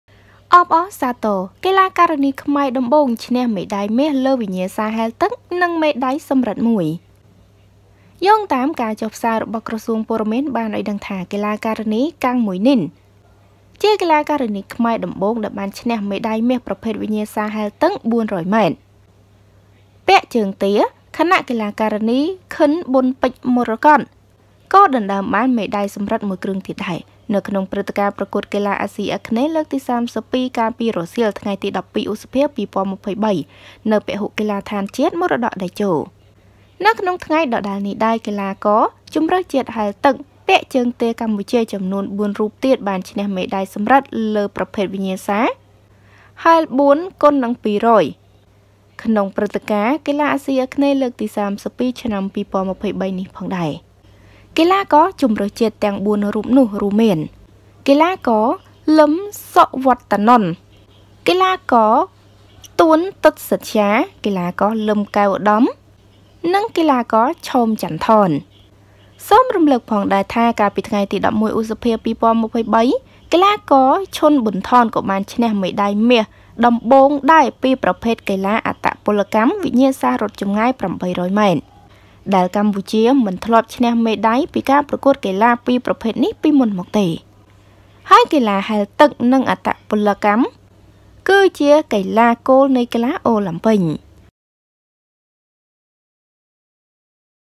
រំភើប!! កីឡាការិនីខ្មែរដំបូង ឈ្នះមេដាយមាសលើវិញ្ញាសាហែលទឹក និងមេដាយសំរឹទ្ធ១
សូមអធ្យាស្រ័យ

ចំពោះការមិនបានតប ខំមិនបានគ្រប់គ្នា

Ms Socheata